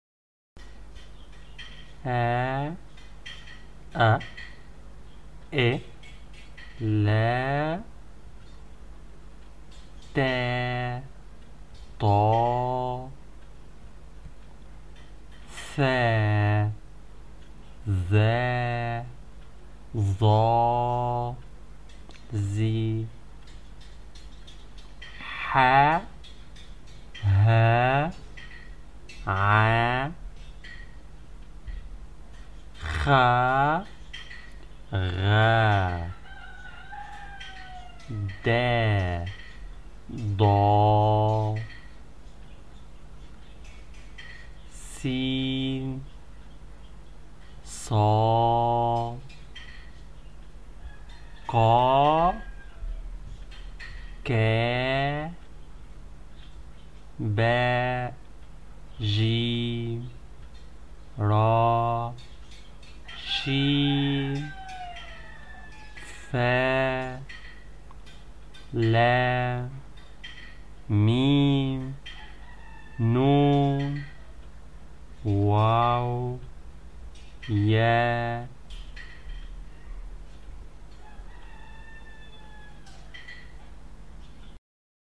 Easiest Way To Understand and Memoriza Arabic Alphabet
Assalamu alikum and welcome to the Arab World.
This lesson is your first step in learning Arabic language. The Arabic alphabet could be challenging for a lot of learners because of many similar sounds, but using this way you will be able to tell apart each sound confidently and know which letter you say or hear.
The alphabet is the foundation of Arabic language. If you mastered letters pronunciation speaking and understanding will be easy.

learn Arabic alphabet for children
Voici  une chanson pour apprendre l'alphabet arabe aux petits